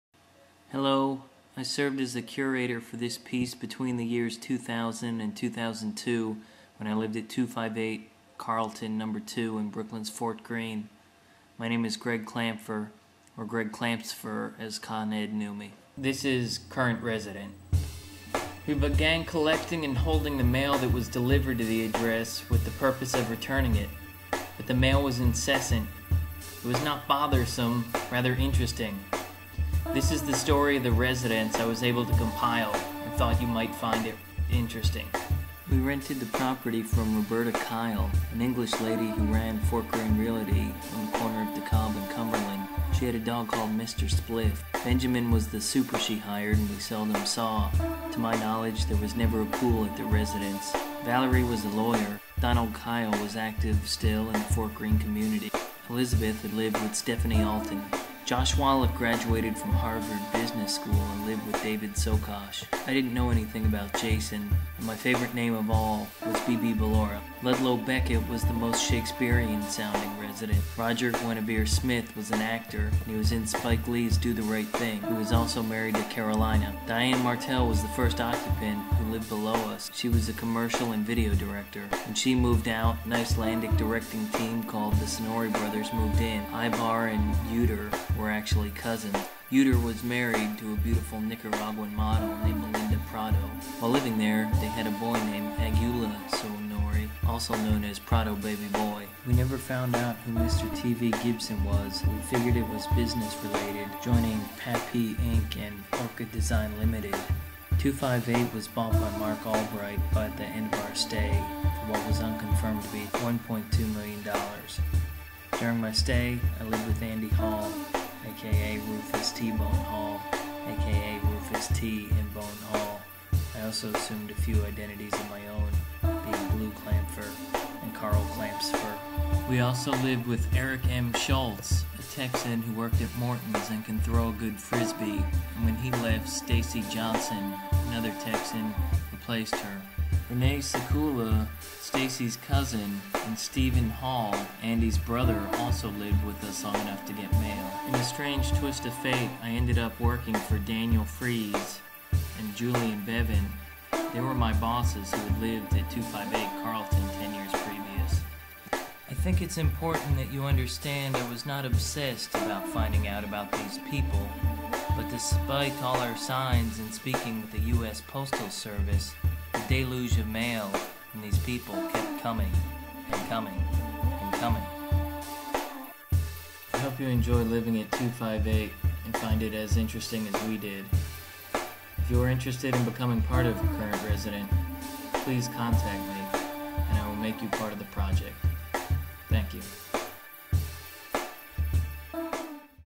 CURRENT_RESIDENT
A film about a postal art piece i curated between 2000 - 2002 in Ft.Green Brooklyn.